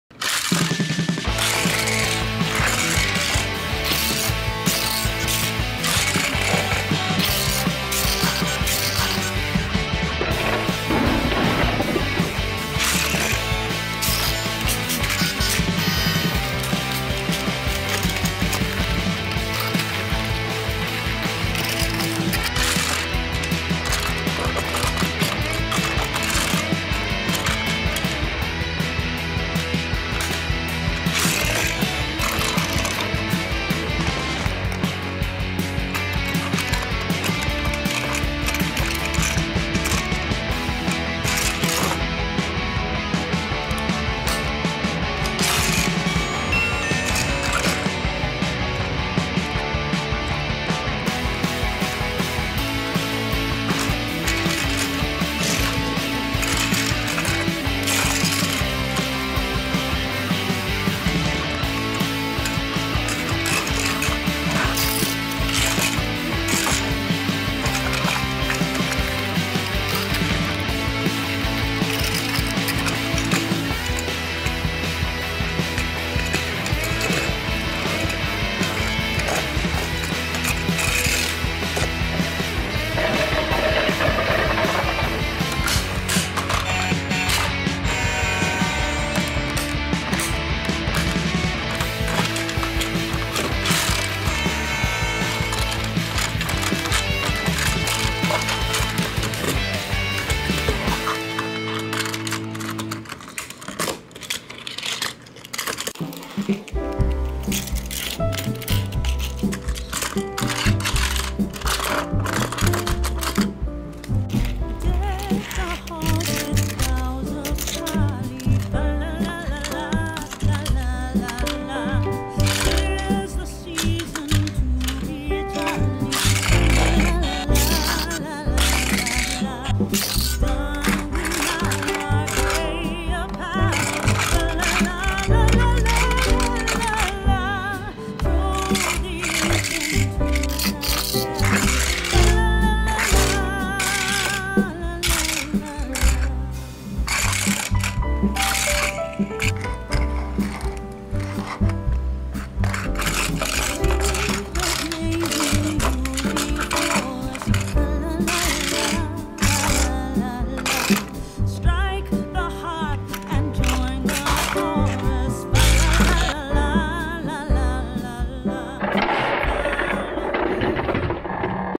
★Disney Pixar's Cars Lightning McQueen & Koko and garage toys★
The Cars toys tried to play here and the garage of Chuggington.
We ran the car in a very fast speed today.

カーズのおもちゃをチャギントンのココと車庫で遊ばせてみました。
今日はとても速いスピードで車が走りました。

Favorite videos